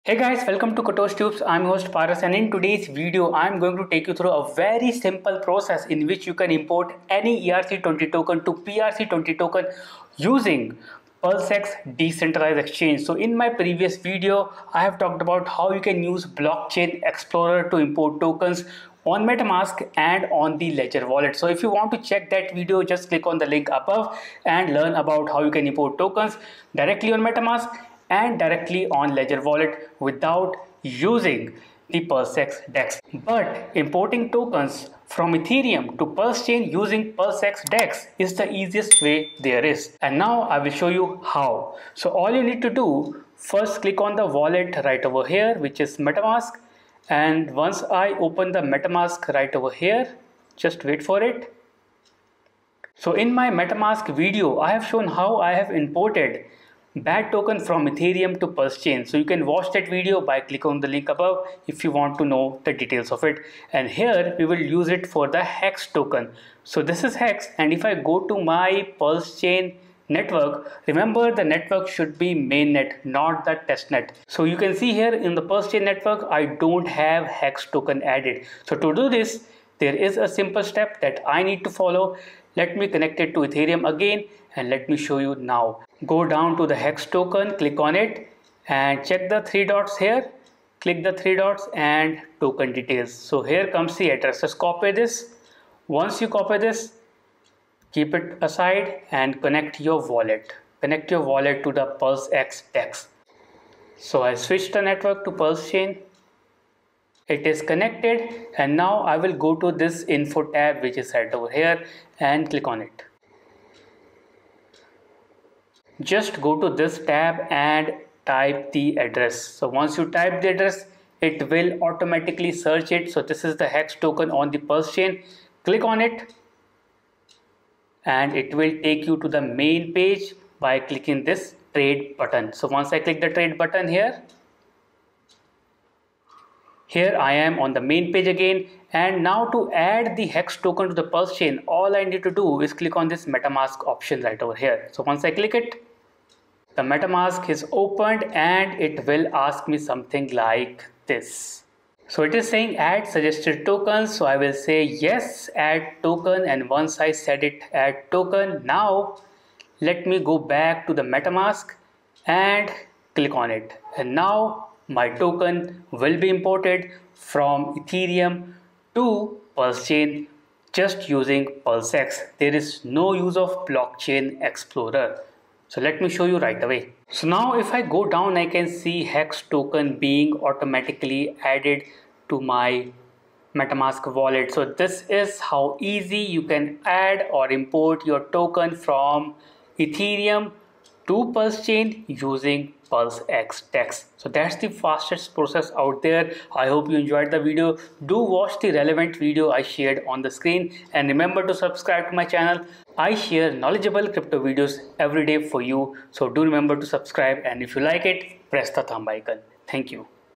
In 2 min. Import ERC20 and HEX Tokens to Pulsechain using PulseX | Tutorial
The easiest way to import Ethereum ERC20 and HEX tokens on Pulsechain mainnet PRC20 in just 2 minutes, Claims airdrops with ease!